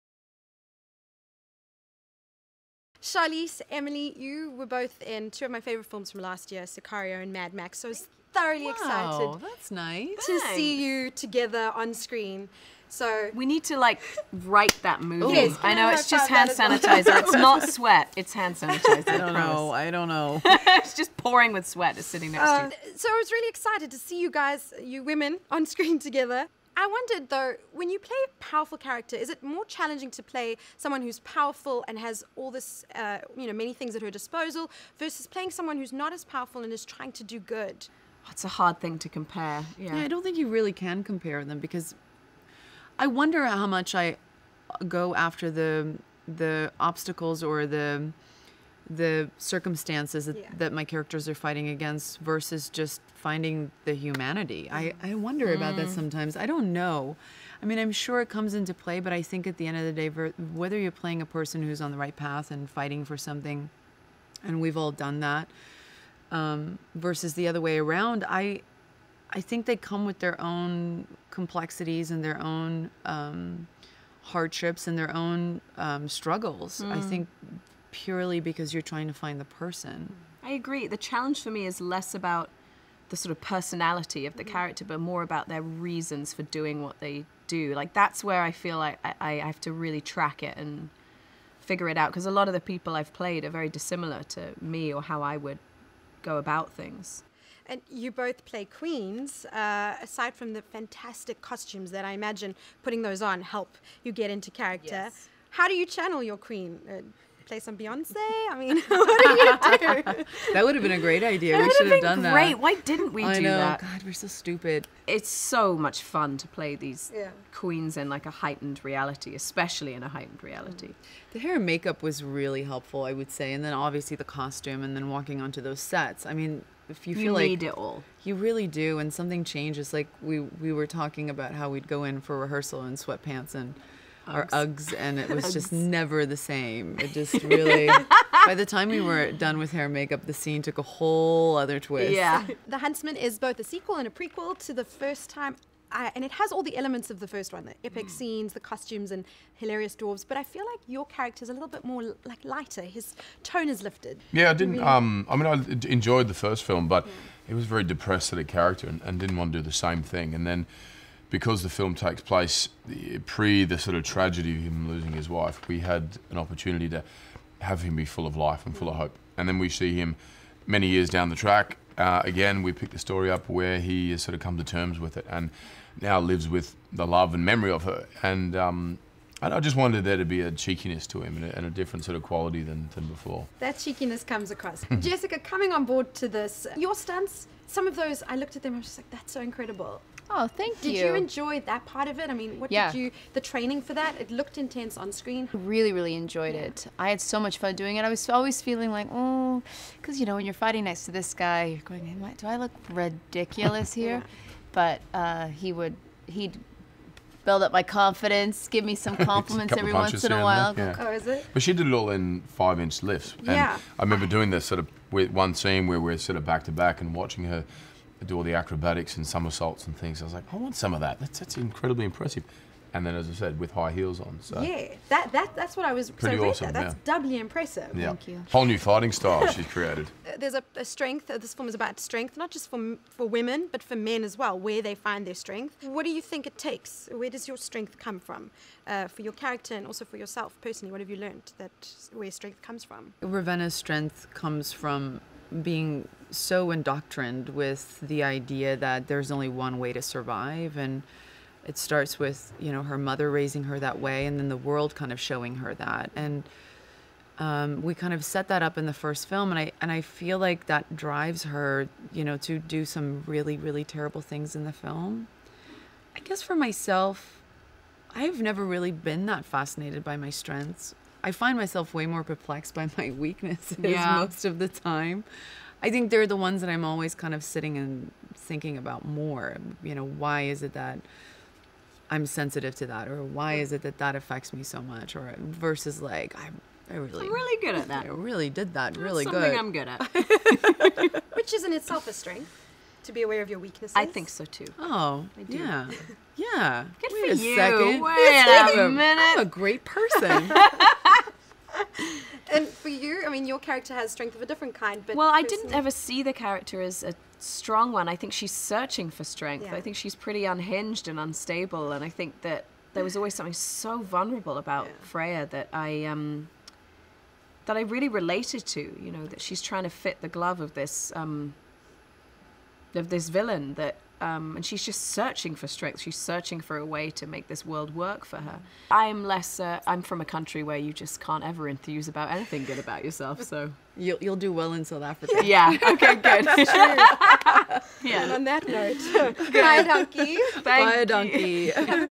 Charlize Theron + Emily Blunt on Strength + Becoming Queens for The Huntsman: Winter's War
I talked to Oscar-winning actress Charlize Theron and Emily Blunt about how they channelled their inner queens, and what strength and power means to them. I also got left out of their high-5 moment (which I created) and spoke to Jessica Chastain about fighting alongside Chris Hemsworth. All in a day's work, really!